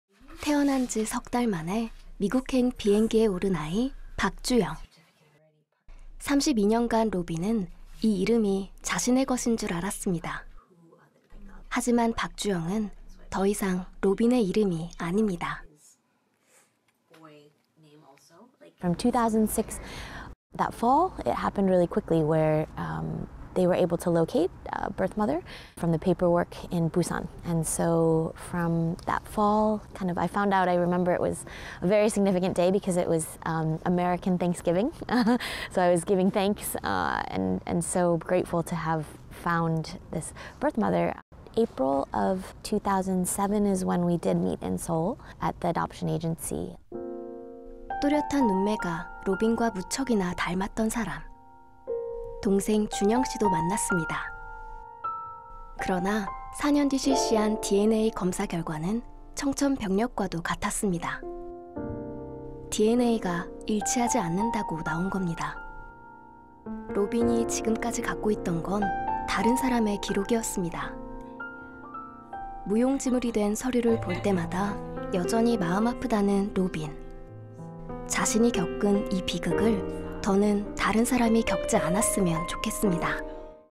입양 서류도 틀릴 수 있나요...입양동포 로빈의 사연 [달려라, 로빈!] / YTN korean
※이 다큐멘터리는 2019년도에 제작되었음을 알립니다.
YTN korean [달려라, 로빈!]

태어난 지 석 달 만에 미국행 비행기에 오른 아이, 박주영

32년간 로빈은 이 이름이 자신의 것인 줄 알았습니다.

하지만, 박주영은 더 이상 로빈의 이름이 아닙니다.

[로빈 조이 박/ 미국 한인 입양인 : 2006년도 가을에 입양 기관에서 제 서류에 명시돼 있던 친모를 찾아냈어요. 아직도 기억해요. 미국은 추수감사절이었는데 제가 하느님께 친엄마 찾아주셔서 감사하다고 했어요. 이듬해인 2007년 4월, 서울에 있는 입양기관에서 친엄마를 만났죠.]

또렷한 눈매가 로빈과 무척이나 닮았던 사람.

동생 준영 씨도 만났습니다.

그러나 4년 뒤 실시한 DNA 검사 결과는 청천벽력과도 같았습니다.

DNA가 일치하지 않는다고 나온 겁니다.

로빈이 지금까지 갖고 있던 건 다른 사람의 기록이었습니다.

무용지물이 된 서류를 볼 때마다 여전히 마음 아프다는 로빈.

자신이 겪은 이 비극을 더는 다른 사람이 겪지 않았으면 좋겠습니다.